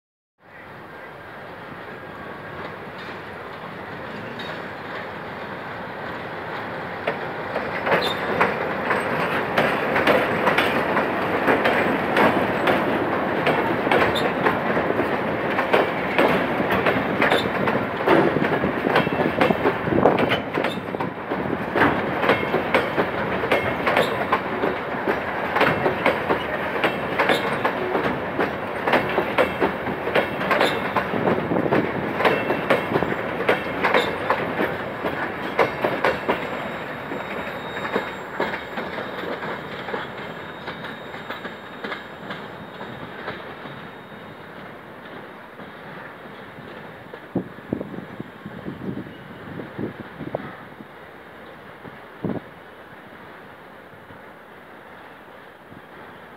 ALCo RSD-16 #8500 + 7 vagones de distinto tipo vacíos de Tablada a Alianza por Haedo
Son los vagones que la 8500 remolcó el lunes con sentido contrario, cargados con arena, hasta la empresa Cristamine, con un depósito ubicado en inmediaciones de la estación Tablada. Con este operativo, no quedaron vagones en ese cliente.

El miércoles 28 de agosto de 2024 a las 12